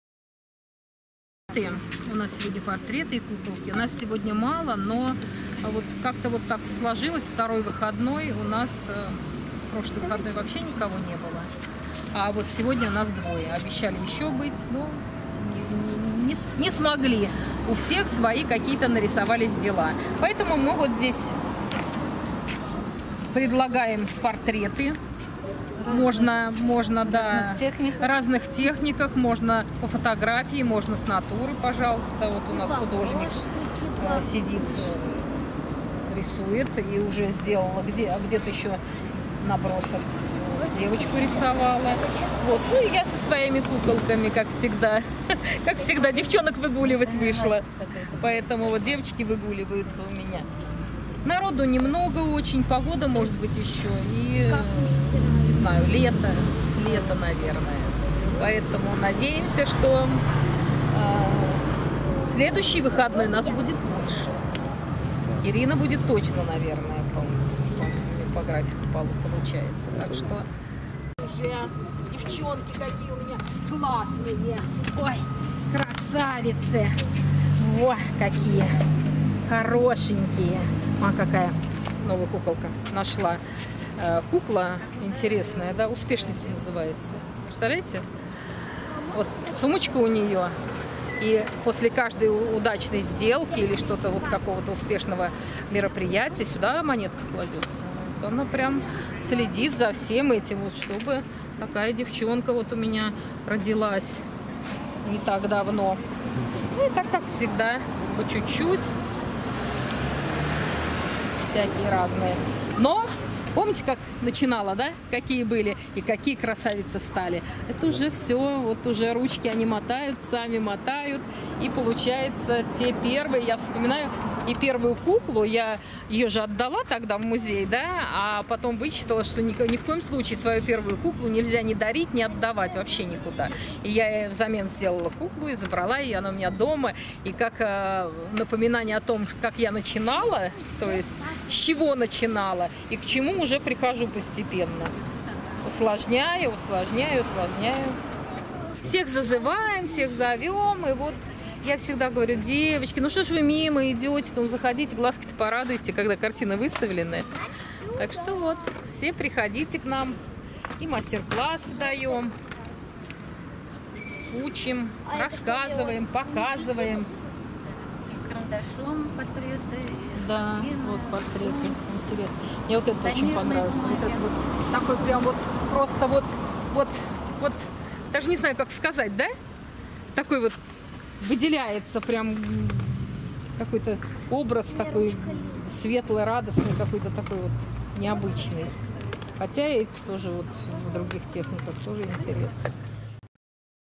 Аллея художников
Каждую субботу на площадке перед ДК им. Ленина работает «Аллея художников. 20 августа г.о. Красноармейск 2016 год